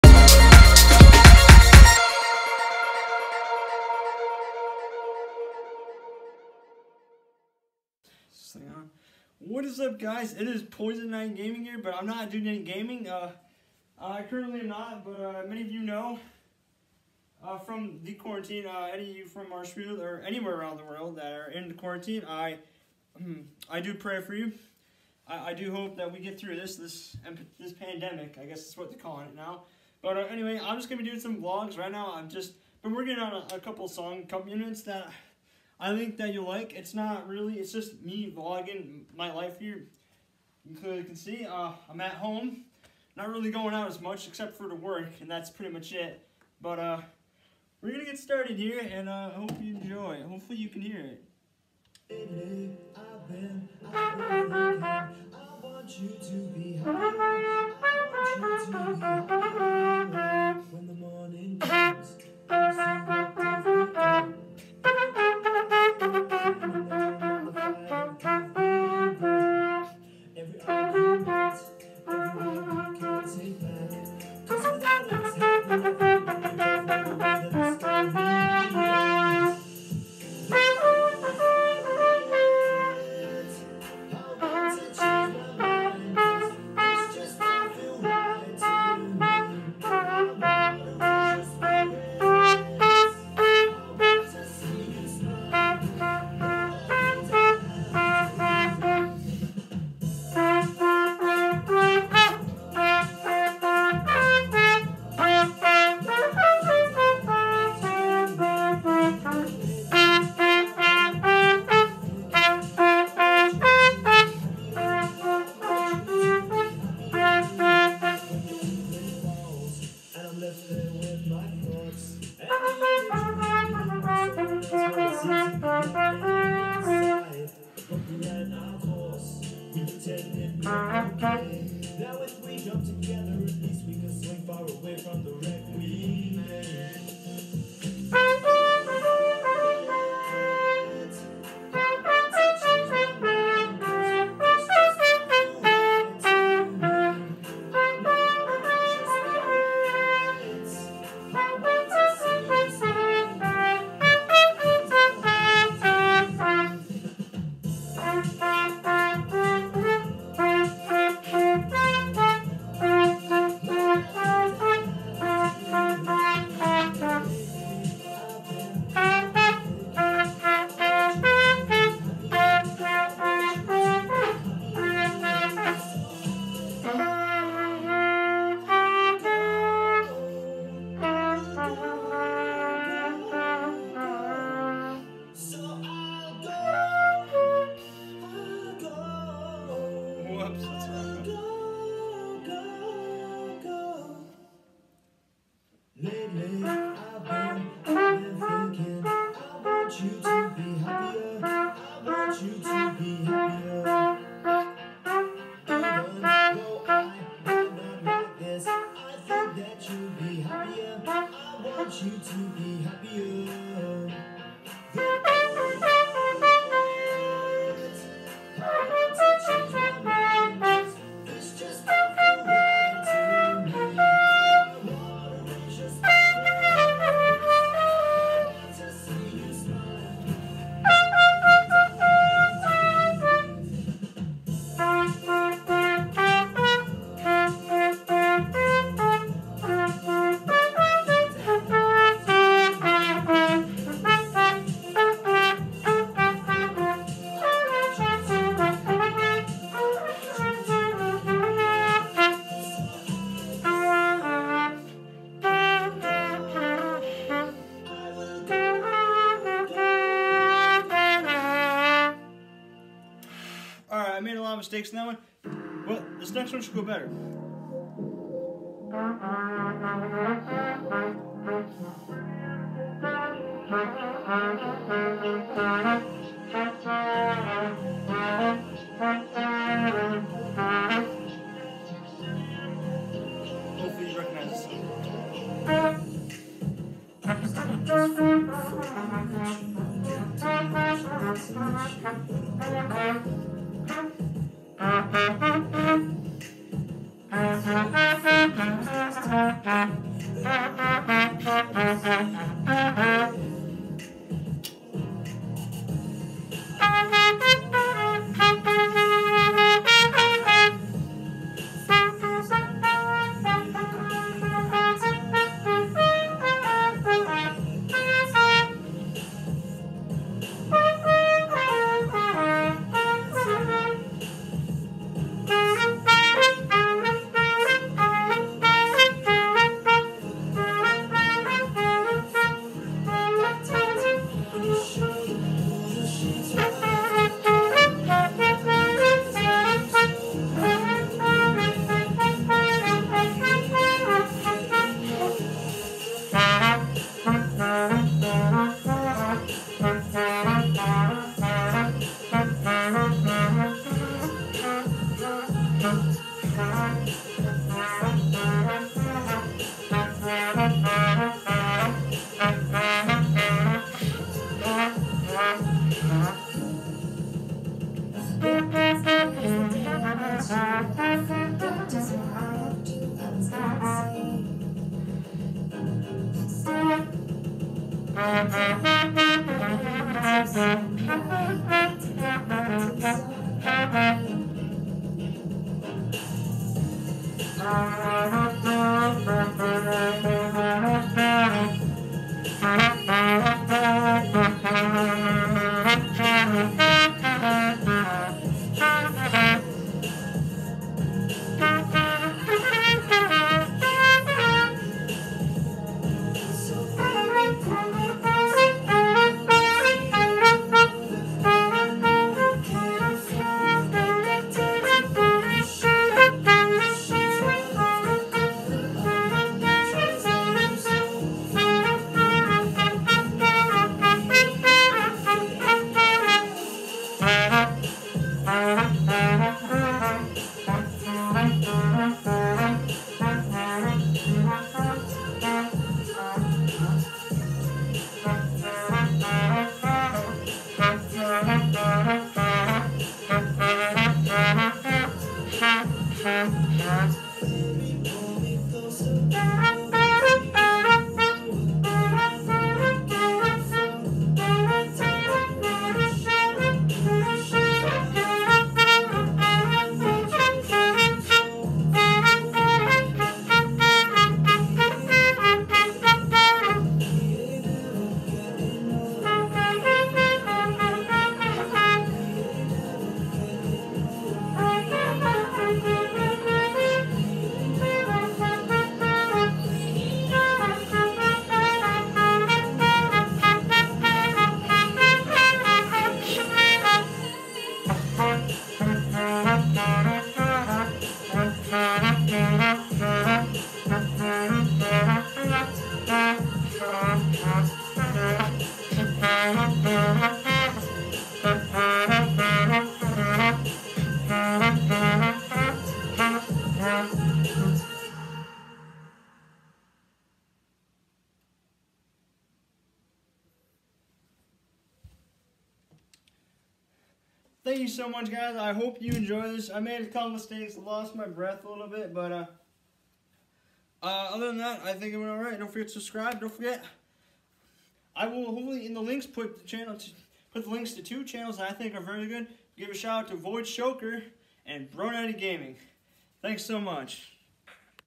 PoisonKnight Vlogs: Trumpet Guy
This video is just a quarantine vlog from my house more will be on the way and maybe some GTA V gaming